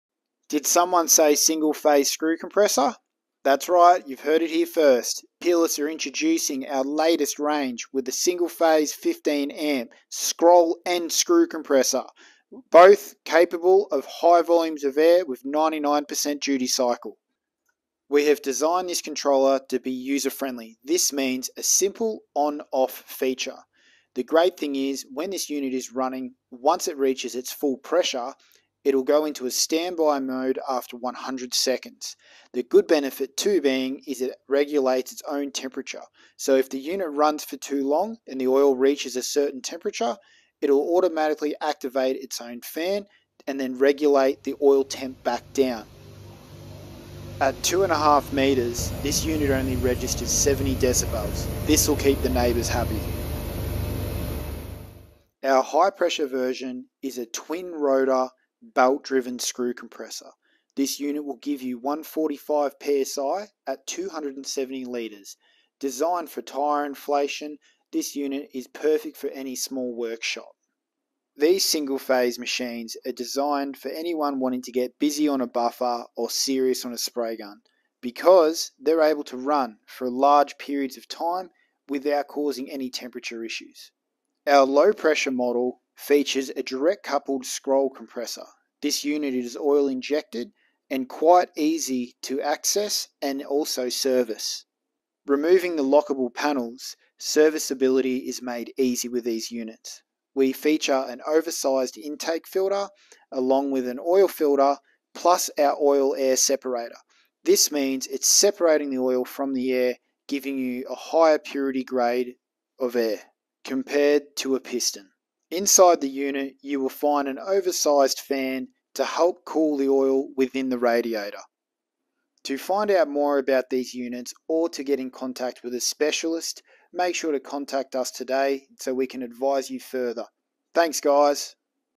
Single Phase Scroll and Screw Compressor 116PSI (8BAR) & 145PSI (10BAR) 15A Plug
3HP Screw & Scroll Single Phase Air Compressors

The PS3/8 Scroll Air Compressor
✔️15 Amp plug
✔️110 Litre Pressure Vessel
✔️320 LPM @116 PSI (8 BAR)

The PS3/10HP Rotary Screw Compressor - High Pressure
✔️15 Amp plug
✔️270 LPM @145 PSI (10 BAR) - 320 LPM @ 116 PSI (8 BAR)

One of the largest Single Phase Air Compressors on the Australian market is here at Peerless Products. Up to a 99% duty cycle making this the ultimate workhorse for your working environment. Well suited for many applications where you need large volumes of air but you don't have 3-phase power or the belt drive air compressor is copping a flogging!

Get in contact with us today to help find your closest dealer!